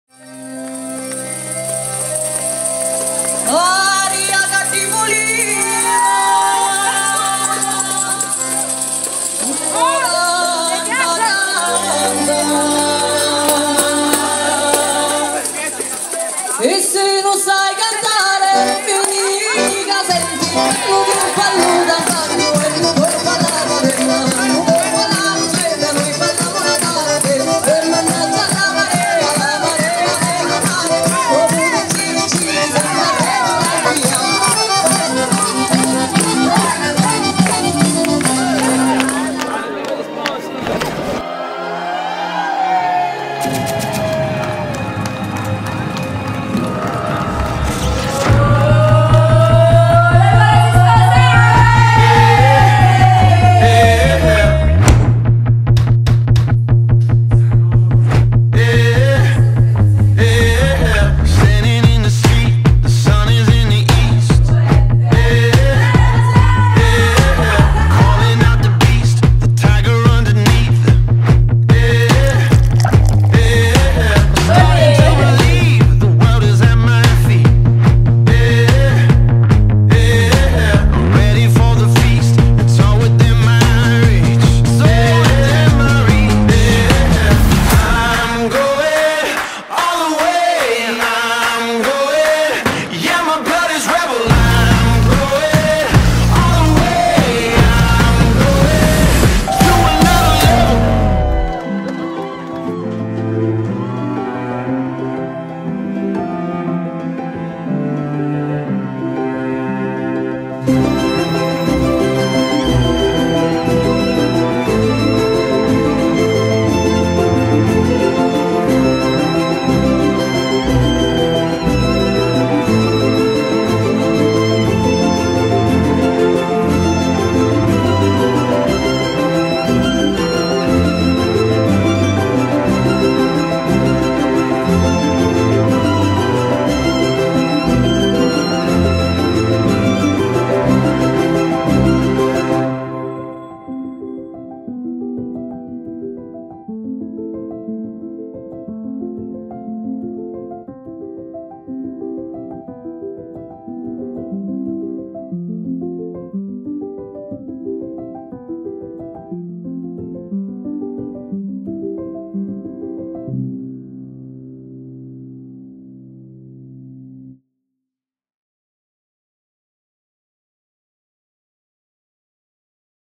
Angelo & Luana Wedding short Film
Una coppia straordinaria, un'intesa pazzesca, una gran voglia di esprimere a gran voce il loro Amore e condividere con tutti la loro Felicità.

Venuti dall'Inghilterra hanno voluto celebrare il loro Matrimonio nella terra dove sono nati, dando vita ad una festa meravigliosa all'insegna delle tradizioni tipiche salentine.